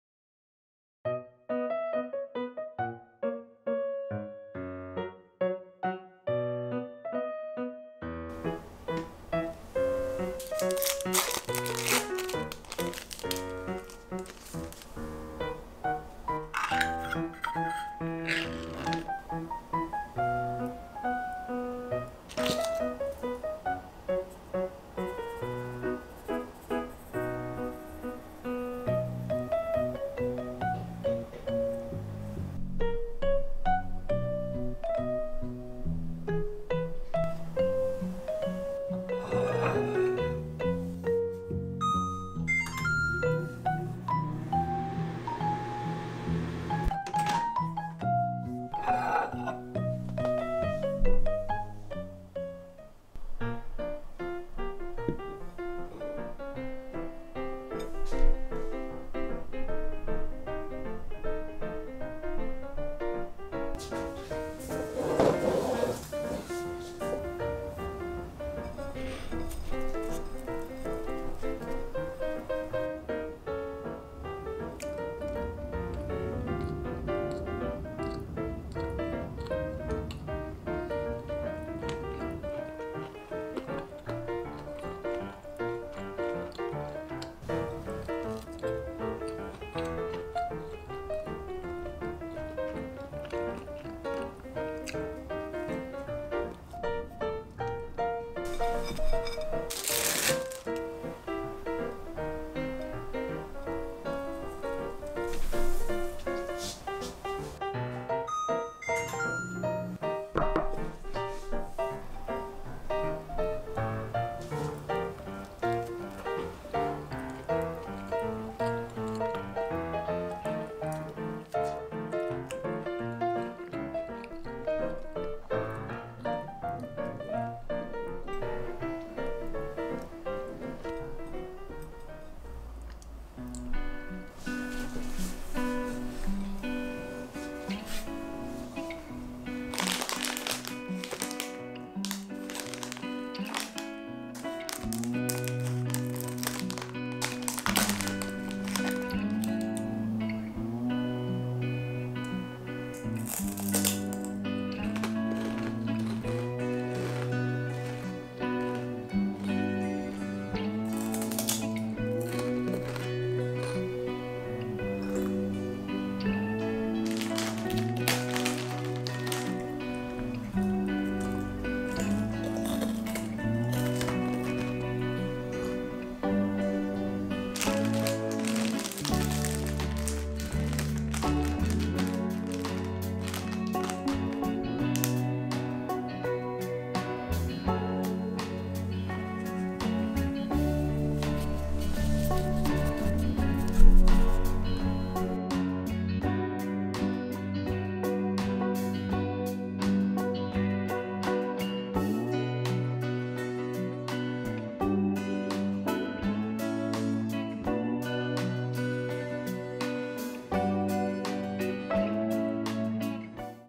눈을 감는 콘치즈빵 Corn Cheese Bread
재료
• 식빵
• 스위트콘
• 슬라이스 치즈
• 설탕

만들기
1. 빵에 치즈, 옥수수, 설탕 뿌려 전자레인지 30~40초 돌린다

✅ 하루한끼 멤버십 대파뱃지 구경가기!